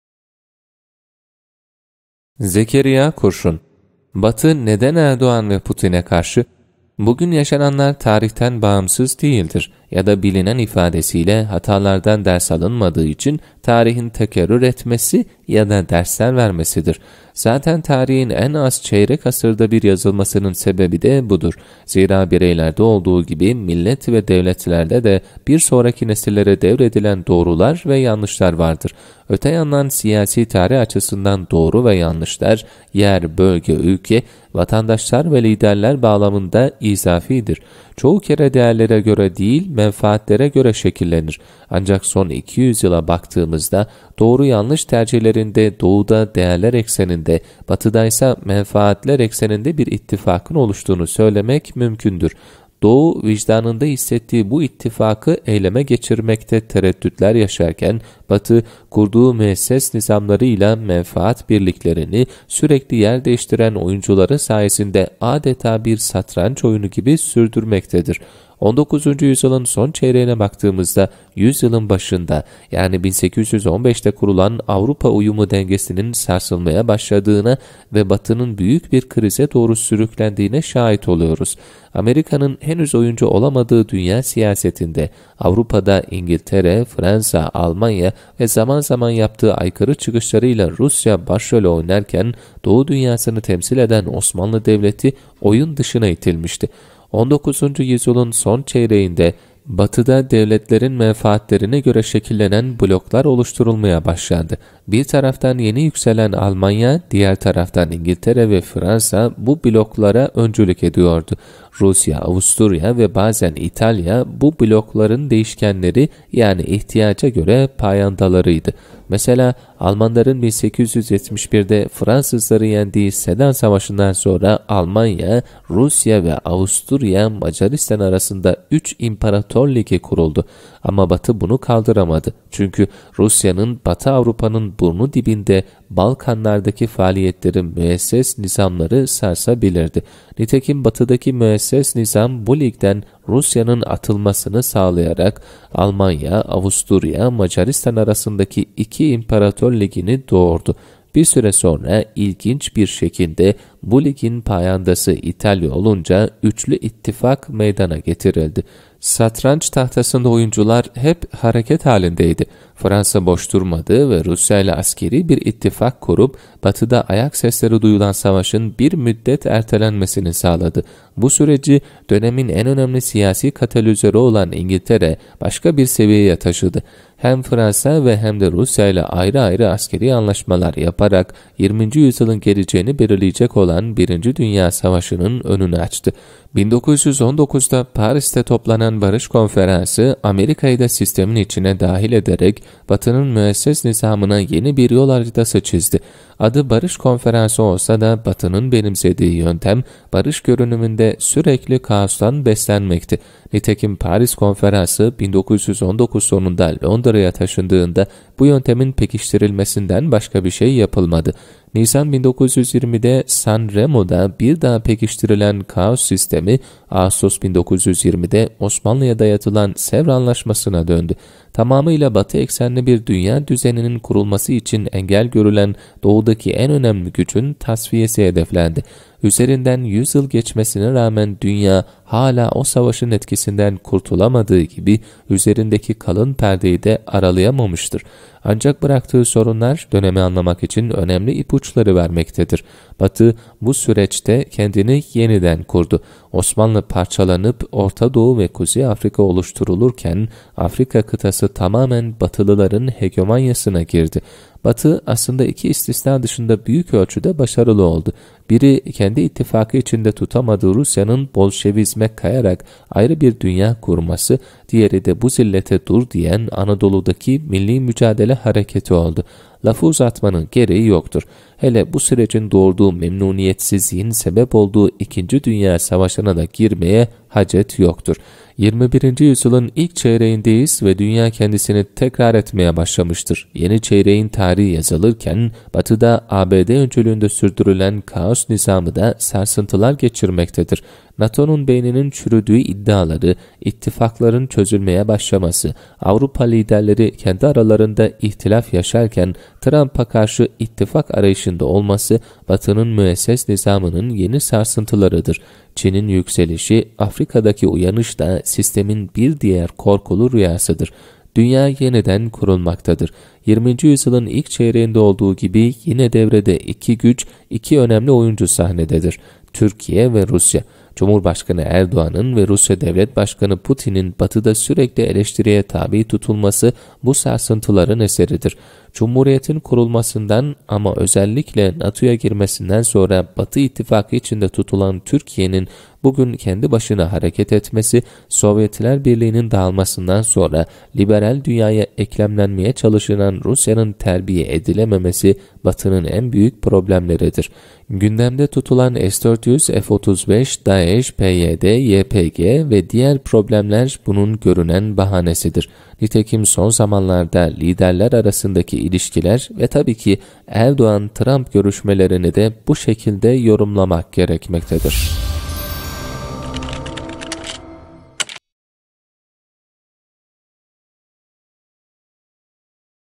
Zekeriya Kurşun - Batı neden Erdoğan ve Putin’e karşı? - 14.11.2019
Bu gün yaşananlar tarihten bağımsız değildir. Ya da bilinen ifadesiyle hatalardan ders alınmadığı için tarihin tekerrür etmesi ya da dersler vermesidir. Zaten tarihin en az çeyrek asırda bir yazılmasının sebebi de budur. Zira bireylerde olduğu gibi millet ve devletlerde de bir sonraki nesillere devredilen doğrular ve yanlışlar vardır. Öte yandan siyasi tarih açısından doğru ve yanlışlar, yer, bölge, ülke; vatandaşlar ve liderler bağlamında izafidir. Çoğu kere değerlere göre değil, menfaatlere göre şekillenir. Ancak son iki yüz yıla baktığımızda, doğru-yanlış tercihlerinde Doğu’da değerler ekseninde; Batı’da ise menfaatler ekseninde bir ittifakın oluştuğunu söylemek mümkündür. Doğu vicdanında hissettiği bu ittifakı eyleme geçirmekte tereddütler yaşarken; Batı, kurduğu müesses nizamları ile menfaat birliklerini, sürekli yer değiştiren oyuncuları sayesinde adeta bir satranç oyunu gibi sürdürmektedir.